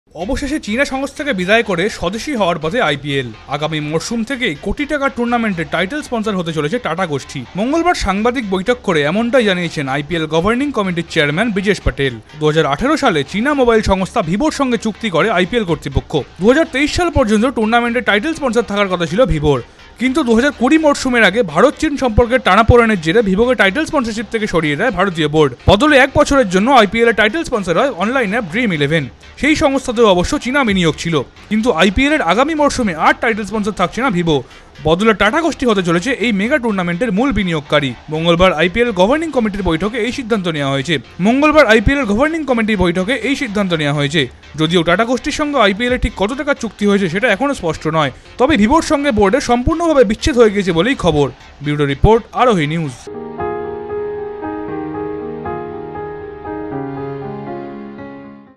ভিভোকে বিদায় জানিয়ে আইপিএলের নতুন স্পনসর টাটাগোষ্ঠী
আইপিএলের নতুন স্পনসর হতে চলেছে টাটাগোষ্ঠী। ২০১৮ সালে চিনা মোবাইল সংস্থা ভিভোর সঙ্গে চুক্তি করে আইপিএল কর্তৃপক্ষ। ২০২৩ সাল পর্যন্ত টুর্নামেন্টের টাইটেল স্পনসর থাকার কথা থাকলেও ভারত-চিন সম্পর্কের টানাপড়েনের জেরে ভিভোকে টাইটেল স্পনসরশিপ থেকে সরিয়ে দেয় ভারতীয় বোর্ড।

@Aarohi New